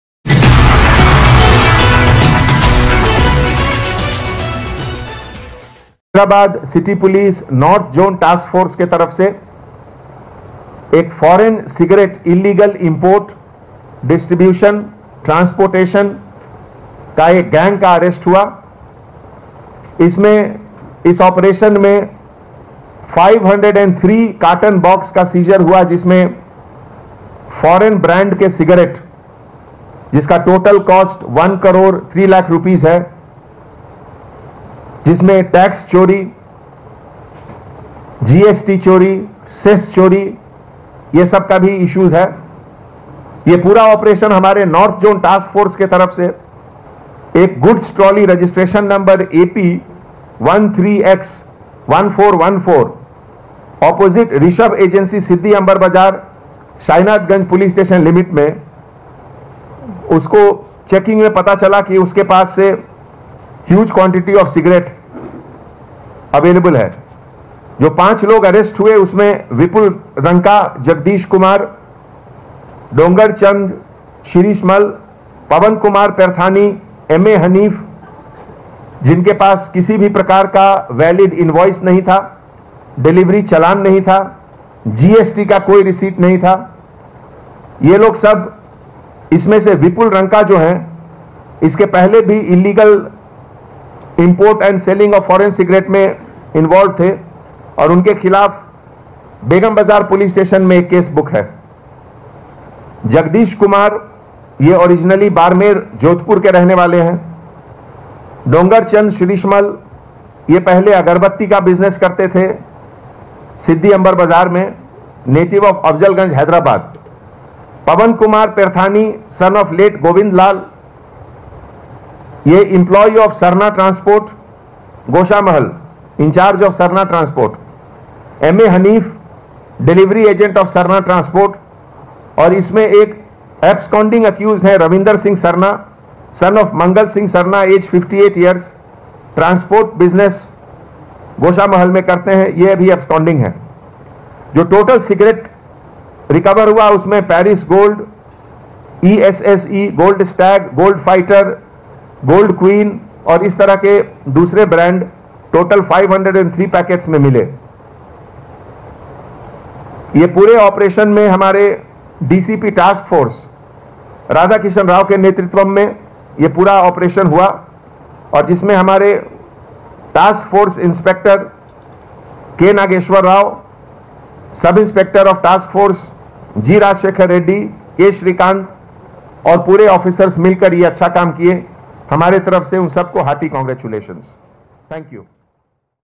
BREAKING NEWS: BUSTED FOREIGN CIGARETTES ILLEGALLY IMPORTS AND SALES RACKET – HELD (05) ACCUSED PERS
BREAKING NEWS: BUSTED FOREIGN CIGARETTES ILLEGALLY IMPORTS AND SALES RACKET – HELD (05) ACCUSED PERSONs GANG -Seized 503 carton box of various brands of foreign Cigarettes all worth about Rs.1,03,00,000/- (One Crore three Lakh rupees) by Commissioner’s Task Force, North Zone team intercepted one Goods Auto trolley Reg.No.AP13X-1414, opposite Rishab Agency, Siddiamber Bazar, Shahinayathgunj PS limits and found a huge quantity of Foreign Cigarettes and on lead proceeded to Sarna Transports Pvt Ltd, Goshamahal, apprehended (05) who are illegal imports the Foreign cigarettes from Delhi to Sarna transport without any valid invoice, delivery Challan, GST invoice and selling to pan shop vendors, Kiran shops, petty vendors in around Hyderabad and gain illegal easy profits.  Details of accused persons:- 1. Vipul Ranka S/o. Veerchand Ranka, age. 42 yrs, Occ. Business (D. Rajesh Kumar& Co, shop Old Feelkhana) R/o. Begum Bazar, Hyderabad, N/o. Rakhi (village), Siwana (Tehsil) Barmer (Dist), Rajasthan State (Illegally imports & selling of Foreign Cigarette) • (Previous the accused arrested in Cr.No.33/2019, U/s. 420 IPC & Sec. 20(2) COTPA Act of Begum Bazar PS) 2. Jagadish Kumar S./o. Gobar Ram, age. 19 yrs, occ. Business R/o. Chaknawadi, Begum Bazar, Hyderabad, N/o. Tutharo Ka Mohalla, Karma was (village), Siwana ( Tehsil), Barmer Dist, Jodhpur Division, Rajasthan State, (Associate) 3. Doongarchand Shrishrimal S/o. Roopchand, age. 65 yrs, Occ. Agarbatthi sales business (Rishabha Agency, Siddiamber Bazar) R/o. Afzalgunaj Hyderabad (Associate) 4. Pavan Kumar Perthani S/o.late Govindlal, age. 42 yrs, Occ. Employee in SARNA Transport, Goshamahal R/o.Basheerbagh, Hyderabad (Incharge of Sarna Transport Pvt Ltd, ) 5. M.A Haneef S/o. M.A. Majeed, age. 40 yrs, occ. An employee in SARNA Transport R/o. Asif Nagar, Hyderabad, (Delivery agent in SARNA Transport service) Absconding Accused:- Ravinder sing Sarna S/o. Mangal Sing Sarna, age. 58 yrs, Occ. Transport business styled as SARNA Transport situated at Goshamahal R/o. Road no.12, Banjara Hills, (Owner of SARNA Transport, Goshamahal) ABSCONDING